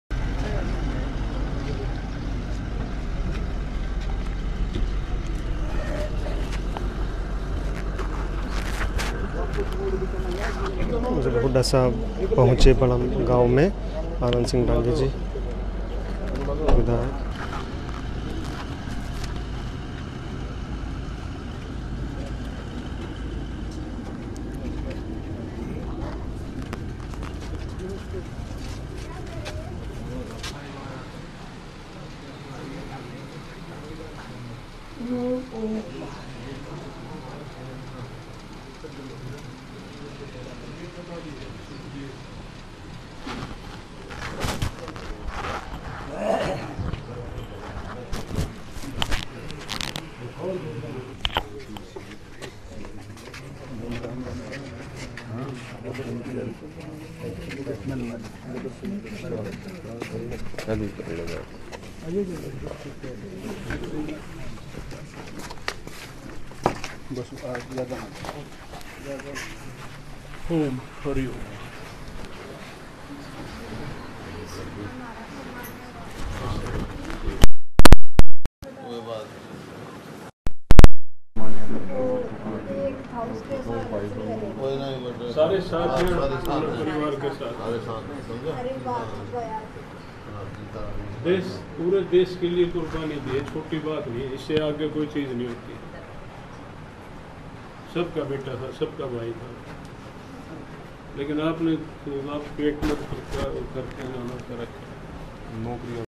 शहीद को श्रद्धांजलि देने पहुंचे पूर्व मुख्यमंत्री भूपेंद्र सिंह हुड्डा हुए भावुक
जब छलक पडे भूपेंद्र सिंह हुड्डा के आंसू !